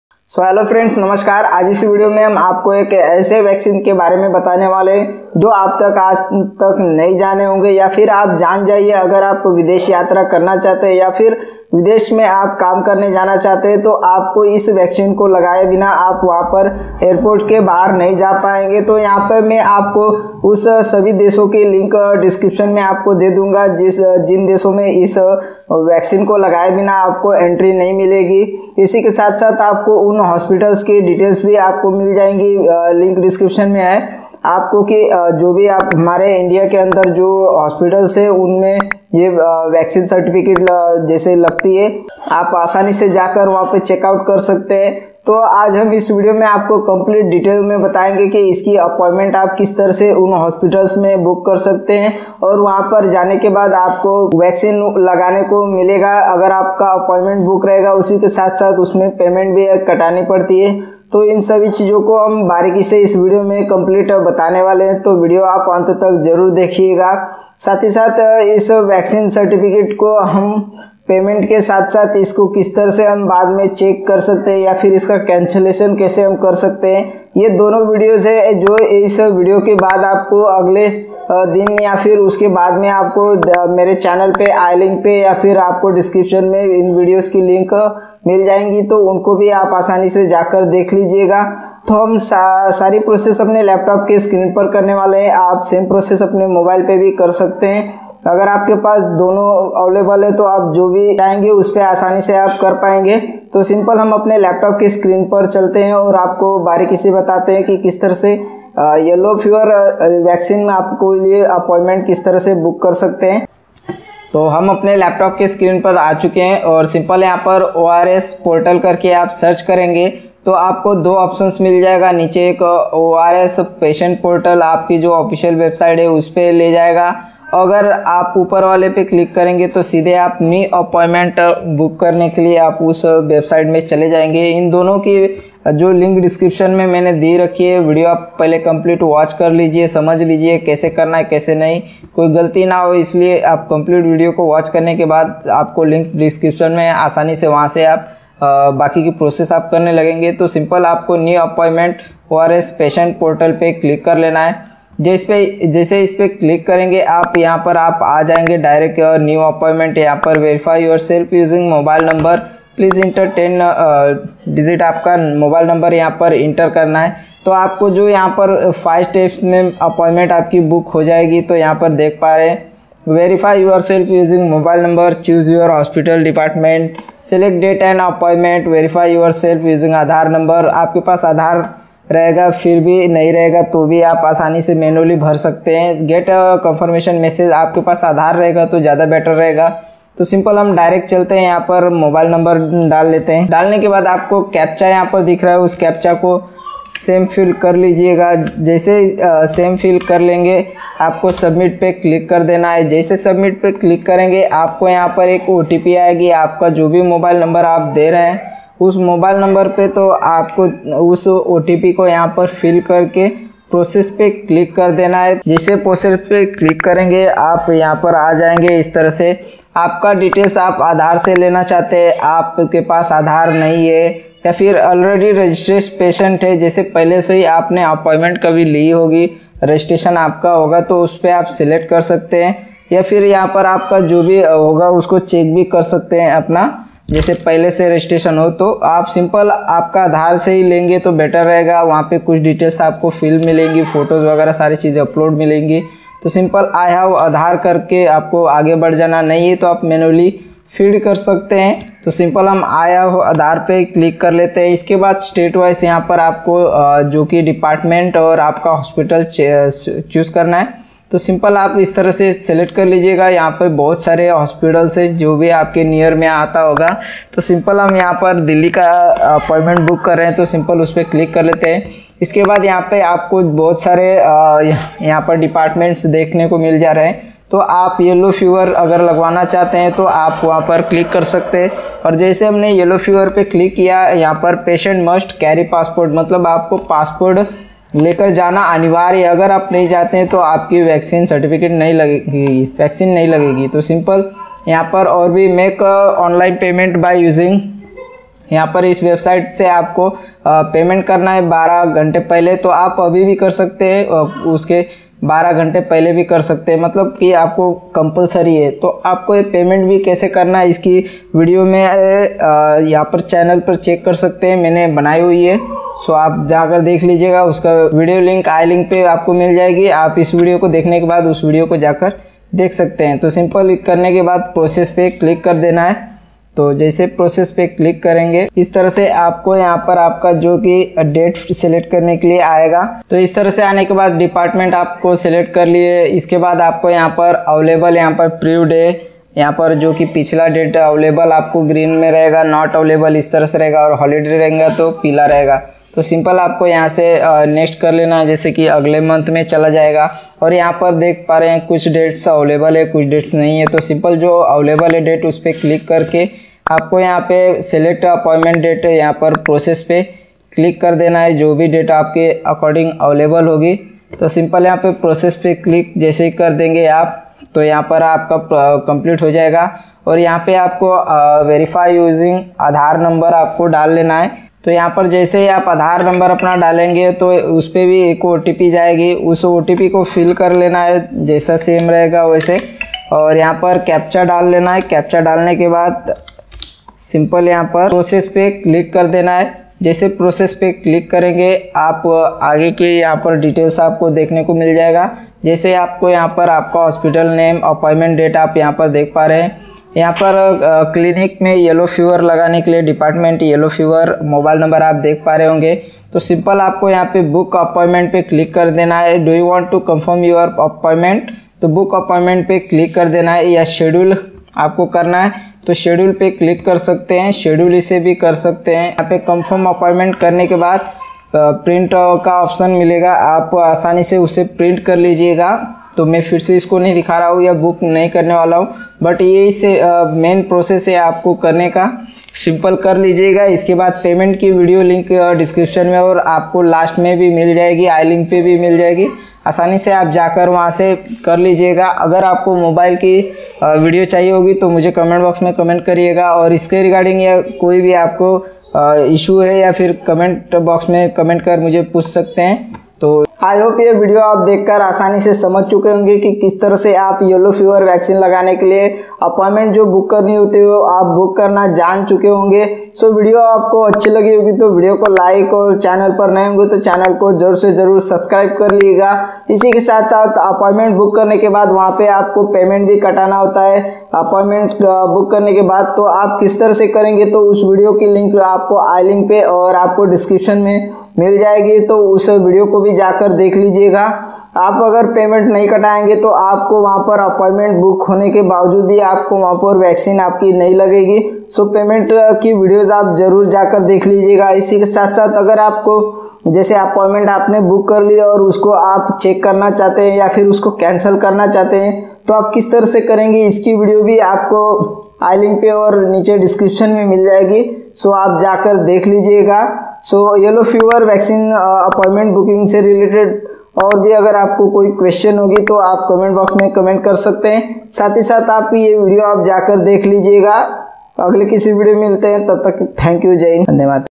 Yellow FEVER vaccine | how to book yellow fever vaccine | yellow fever vaccine online appointment
Technical Rampravesh

Video Details :-
Aaj Hum Is Video Me Sikhenge Ki Kiss Tarah Se Hum Yellow Fever Vaccine Lagane Hetu Kiss Tarah se Oppointment Book Kar Asani Se Hum Jiss Hospital me Oppointment lekar Yellow Fever Vaccine Laga sakte HAi
our Vaccine Lagane ke sath Aap agar our Kissi Emergancy me bhi Online Is video Ko Dekh Online Oppointment Booking Karna Jaan Jayenge .

Online Appointment Booking Documents
1 - Mobile Number
2 - aadhar card
4 - Passport
5 - any Payment Mode 300 RS

Others Important Links :-

how to book yellowstone lodgingyellow fever kaise hota hai